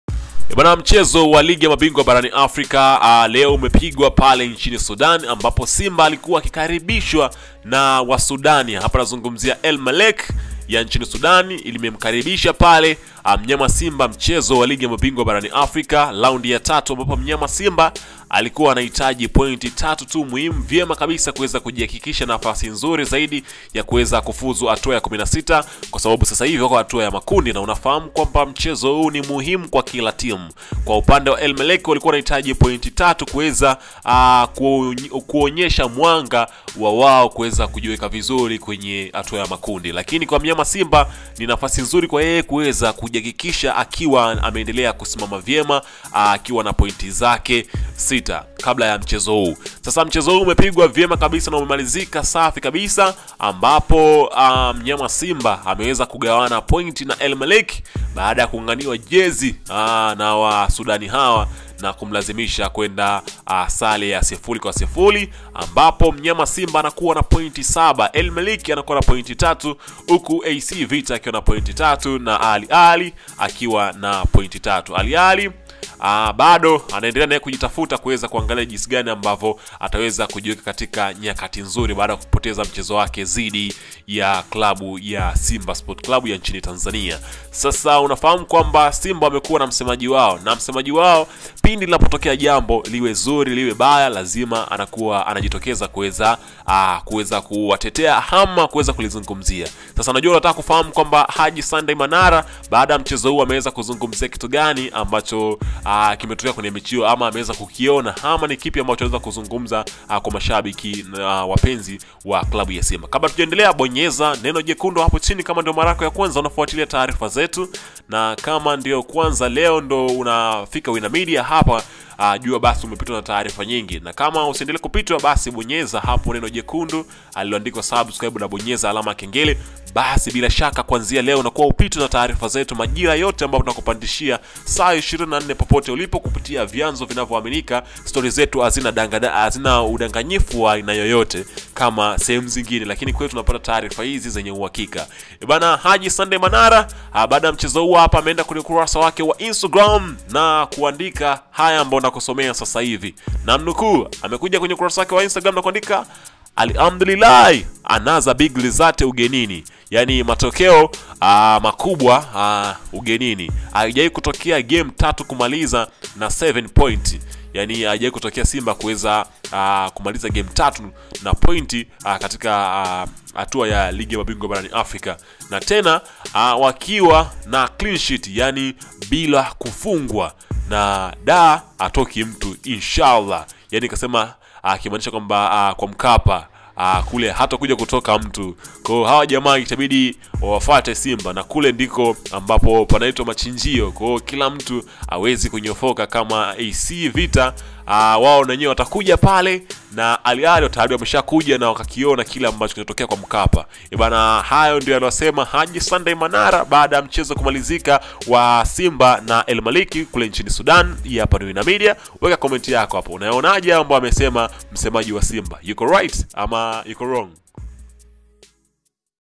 Alichokisema Haji Manara Baada Ya Simba Sc Kutoa Sare Na El Mallekh "Hii Haijawai Kutokea Kabisa"
Alichokisema HajiManara Baada Ya SimbaSc Kutoa Sare Na ElMallekh "Hii Haijawai Kutokea Kabisa"